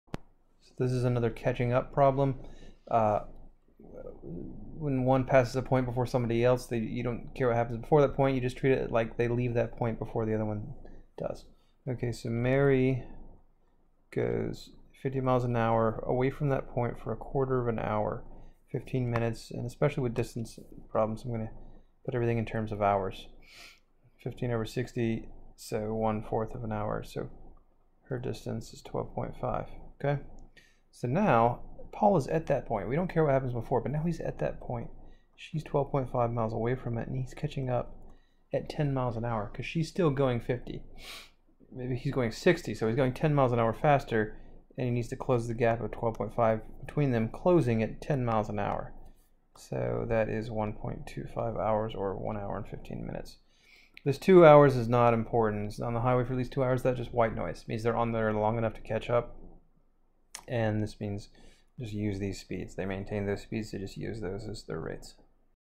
Distance 8- Mary passed a certain gas station on a highway while traveling west at a constant...
Mary passed a certain gas station on a highway while traveling west at a constant speed of 50 miles per hour. Then, 15 minutes later, Paul passed the same gas station while traveling west at a constant speed of 60 miles per hour. If both drivers maintained their speeds and both remained on the highway for at least 2 hours, how long after he passed the gas station did Paul catch up with Mary?

My name is Dan and I offer elite, pay-as-you-go GMAT and GRE tutoring in NYC and online.